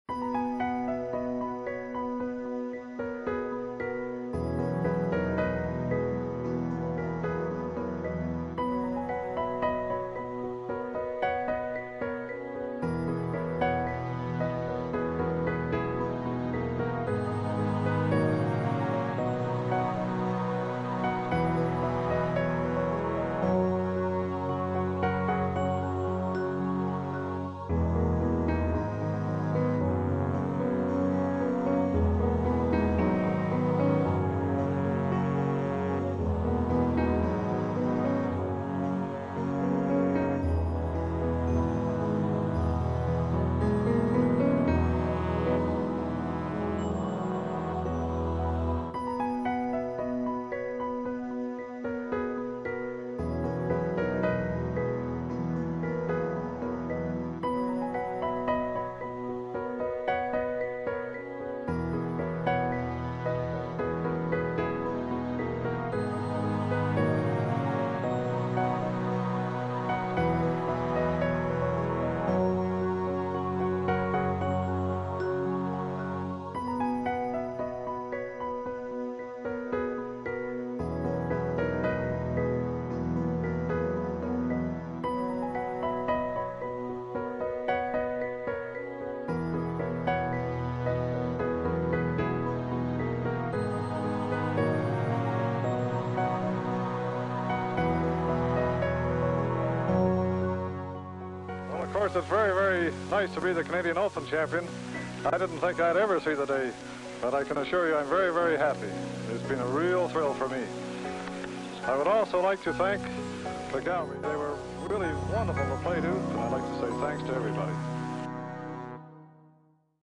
1954 Canadian Open highlight
The last Canadian to hoist the Canadian Open trophy was Pat Fletcher of Saskatchewan at the 1954 Canadian Open held at Point Grey Golf and Country Club in British Columbia.  Beating out another Canadian, Gordon Brydson, Fletcher wins with a score of 280 and $3,000.